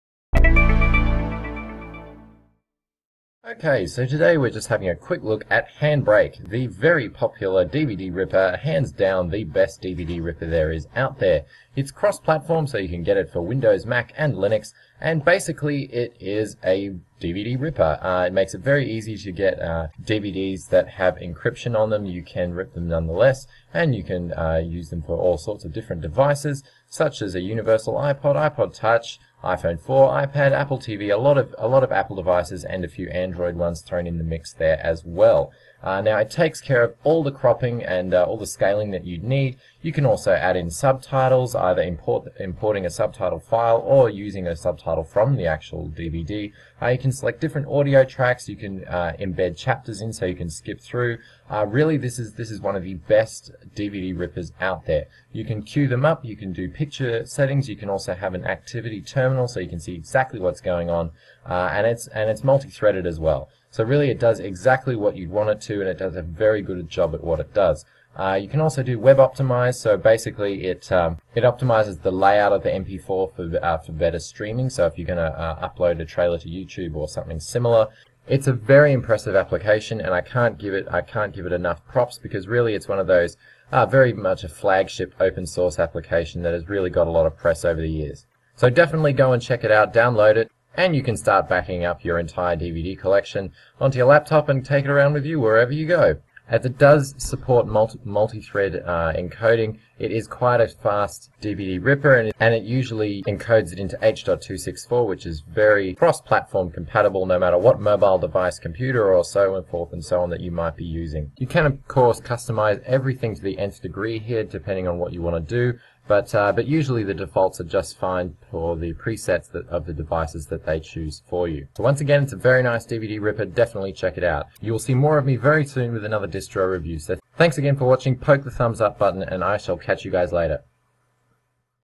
Handbrake Review - App Reviews
A quick review of Handbrake DVD Ripper. Definitely the best DVD ripper out there that is multithreaded, device ready, and cross-platform.

Also available in most community repositories for Linux distributions.